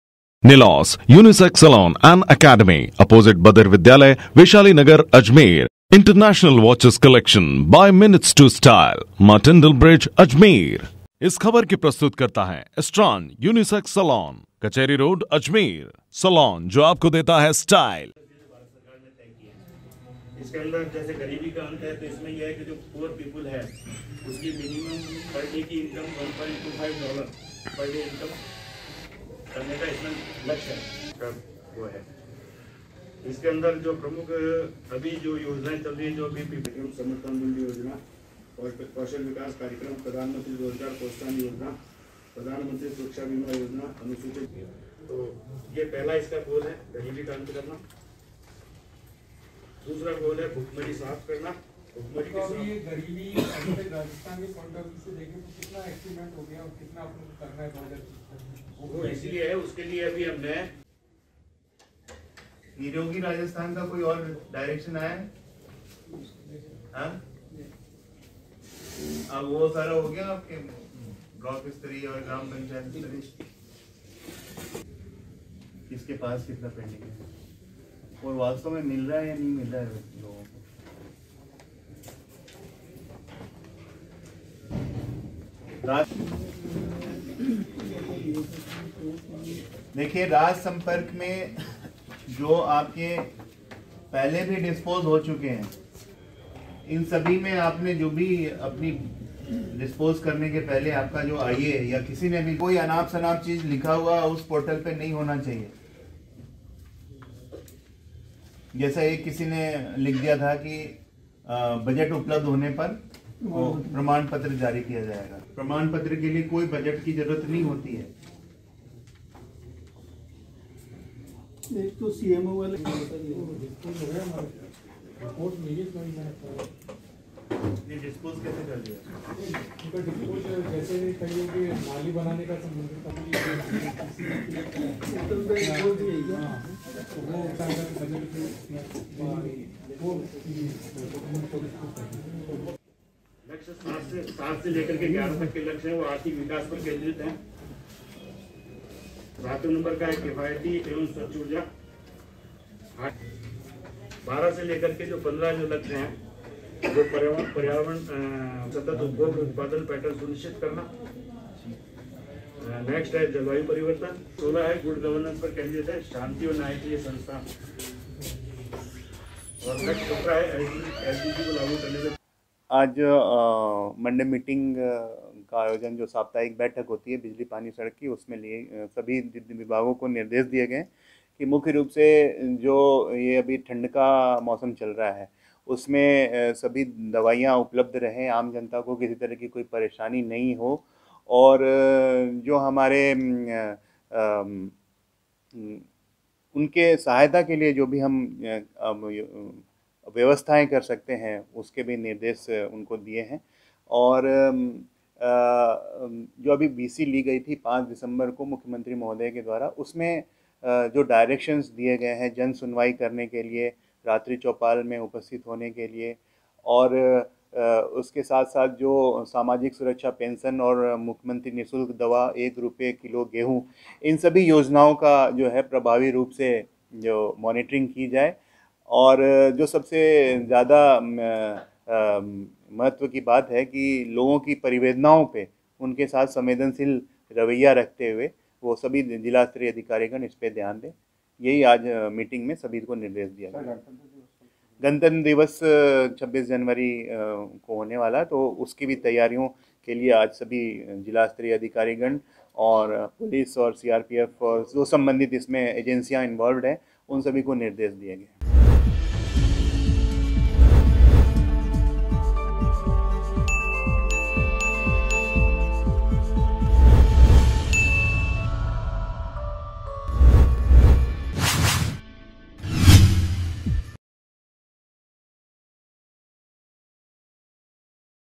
AJMER NEWS | जिला कलेक्ट्री में सोमवार को हुई मीटिंग | MTTV INDIA
जिला कलेक्ट्री में सोमवार को हुई मीटिंग
जिला कलेक्टर विश्व मोहन शर्मा ने दिए दिशा-निर्देश
सरकारी योजनाओं को जिले में प्रभावी रूप से लागू करने के लिए दिए थे मुख्यमंत्री ने दिशानिर्देश
मीटिंग में 26 जनवरी से जुड़ी तैयारियों की भी की गई चर्चा